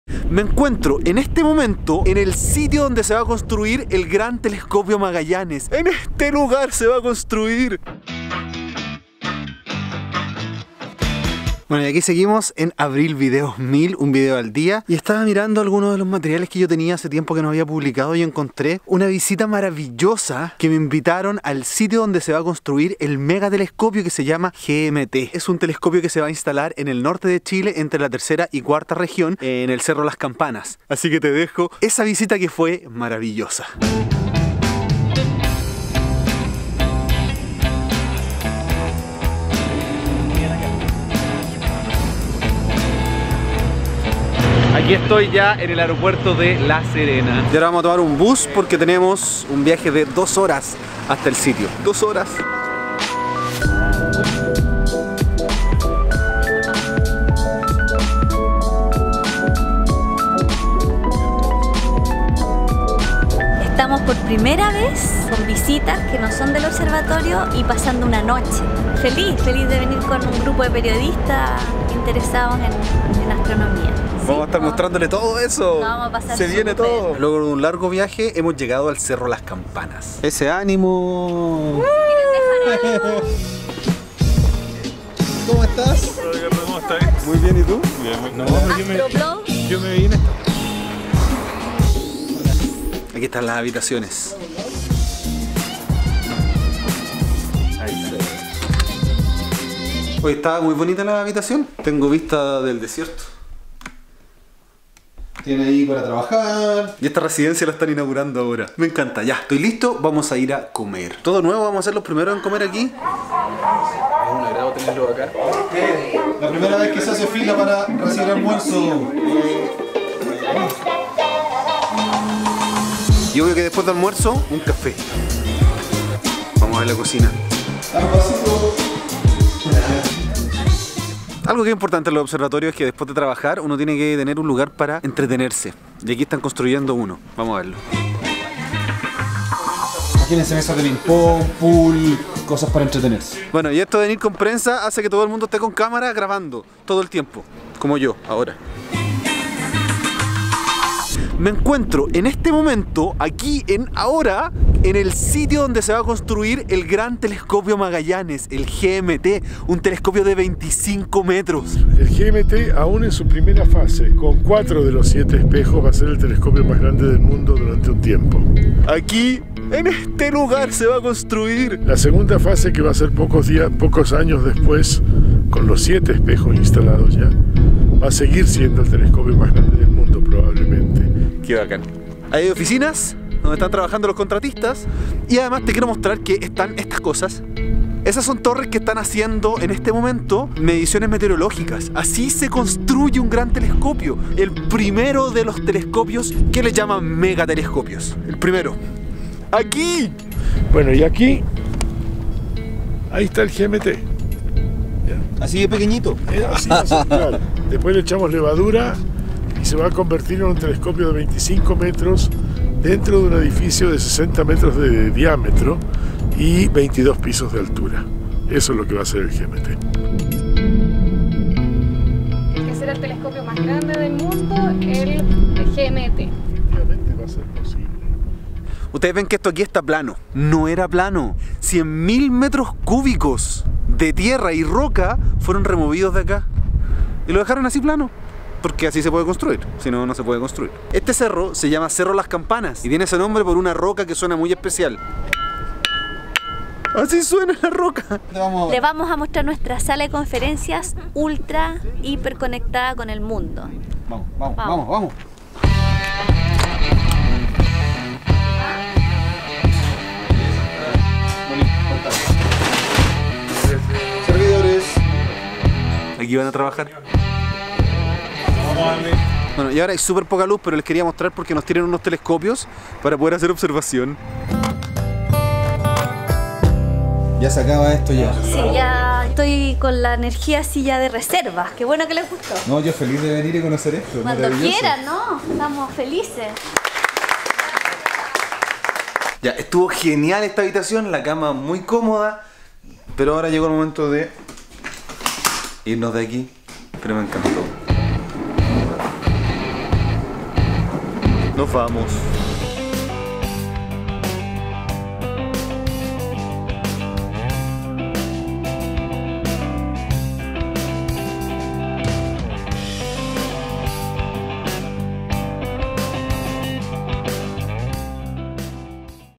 GMT: Así se construye un MEGA-TELESCOPIO
VLOG7.
Aunque lo publico un día más tarde aquí está, la visita al sitio del futuro telescopio GMT que por algunos años será el más grande del mundo.